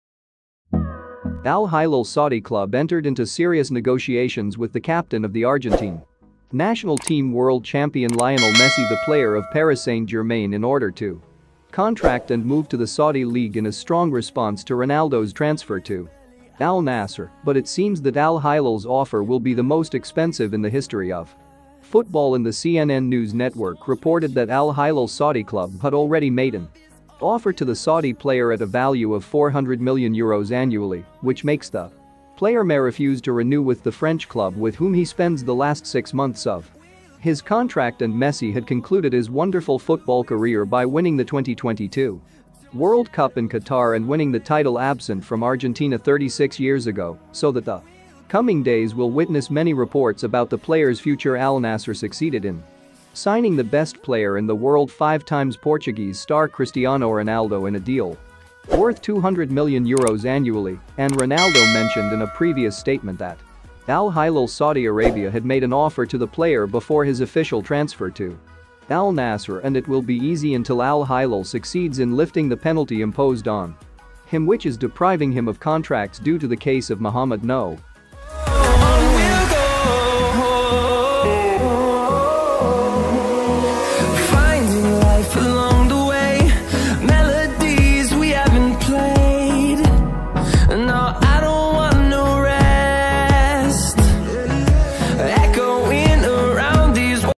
A fictional offer from the Saudi Crescent to Messi .. the most expensive in the history of football
Al-Hilal Saudi Club entered into serious negotiations with the captain of the Argentine national team, world champion Lionel Messi, the player of Paris Saint-Germain, in order to contract and move to the Saudi League in a strong response to Ronaldo’s transfer to Al-Nasr, but it seems that Al-Hilal’s offer will be the most expensive in the history of football. And the CNN news network reported that Al Hilal Saudi Club had already made an offer to the Saudi player at a value of 400 million euros annually, which makes the player may refuse to renew with the French club, with whom he spends the last 6 months of his contract. And Messi had concluded his wonderful football career by winning the 2022 World Cup in Qatar, and winning the title absent from Argentina 36 years ago, so that the coming days will witness many reports about the player’s future. Al-Nasr succeeded in signing the best player in the world 5 times, Portuguese star Cristiano Ronaldo, in a deal worth 200 million euros annually, and Ronaldo mentioned in a previous statement that Al-Hilal Saudi Arabia had made an offer to the player before his official transfer to Al-Nasr. And it will be easy until Al Hilal succeeds in lifting the penalty imposed on him, which is depriving him of contracts due to the case of Muhammad Kno